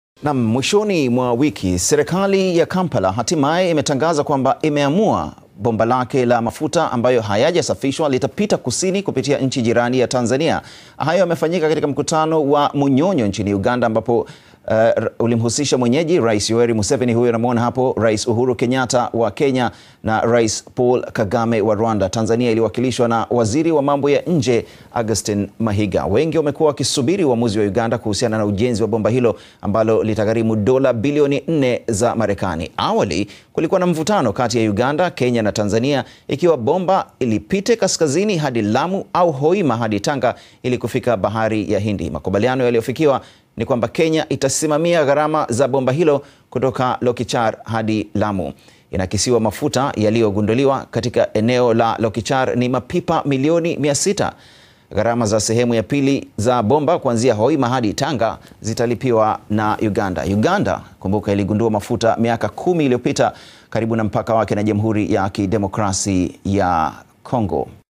Bomba la mafuta ya Uganda kupitia Tanzania
Baada ya vuta nikuvute kuhusu ujenzi wa bomba la kusafirisha mafuta ghafi kutoka Uganda hadi Bahari ya Hindi, Uganda mwishoni mwa wiki iliamua kwamba mafuta yake yatasafirishwa kupitia bandari ya Tanzania, Tanzania